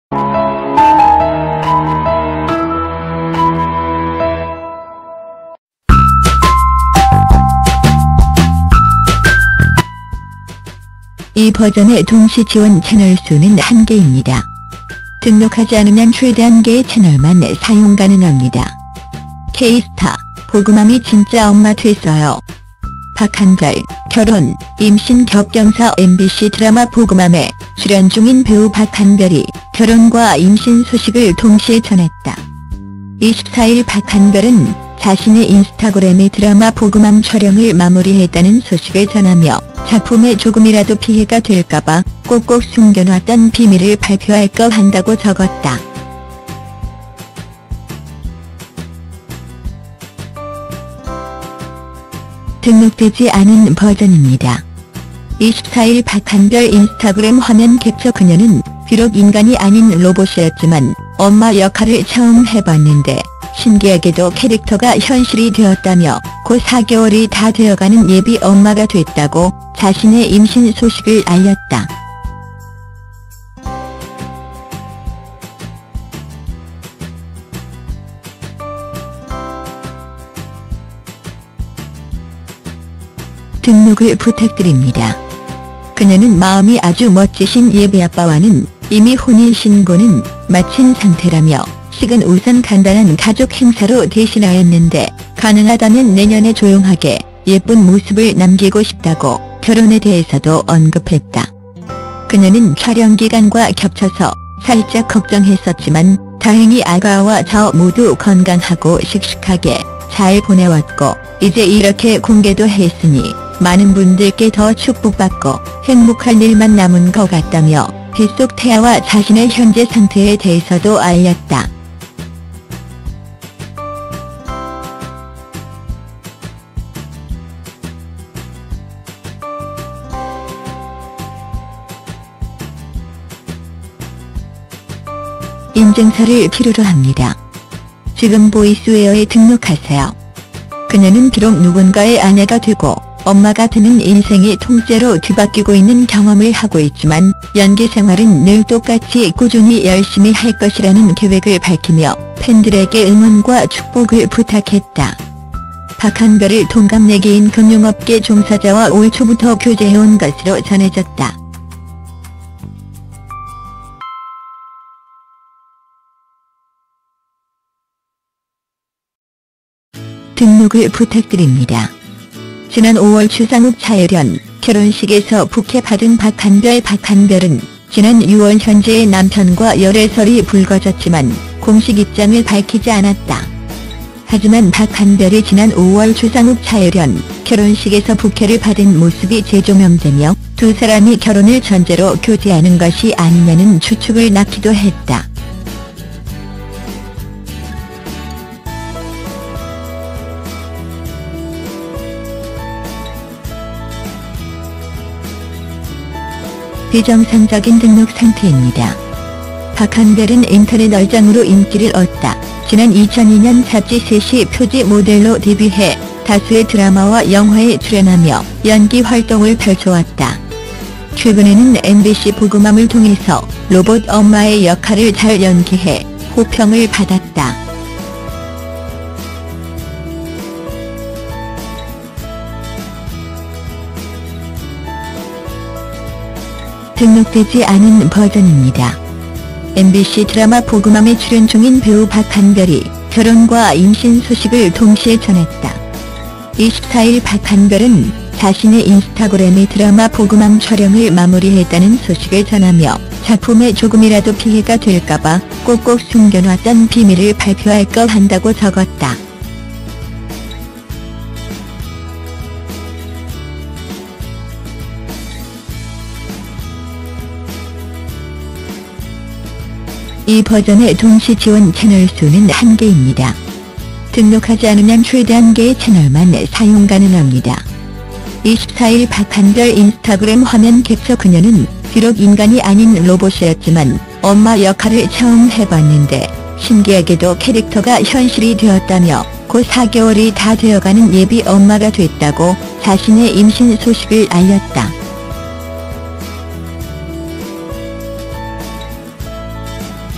[K스타] 보그맘이 진짜 엄마 됐어요...박한별 결혼·임신 겹 엔터테인먼트
뮤뱅 워너원 방송출연 없이 1위 3관왕..올킬 행진[종합]  댓글 [ 엔터테인먼트: